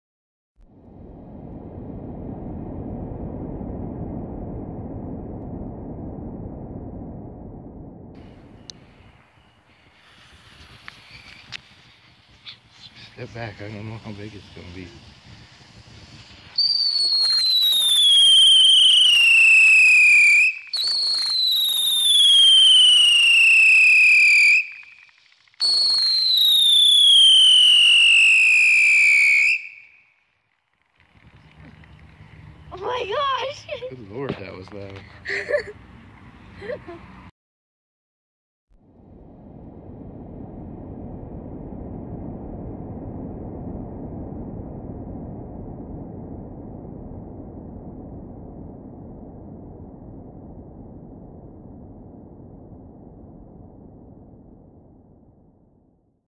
Ninja Stars Shuriken By Cannon Brand (Super Loud Whistling Ground Spinner)
We're checking out Ninja Stars Shuriken by Cannon Brand Fireworks! Talk about bang for your buck! This little firework has it all! Spins on the ground, whistles/screams and colored crackle effects! If you see this one, Buy It!

Type = Ground Spinner With Crackle & Whistle/Screaming!
Total Burn Time = 14 seconds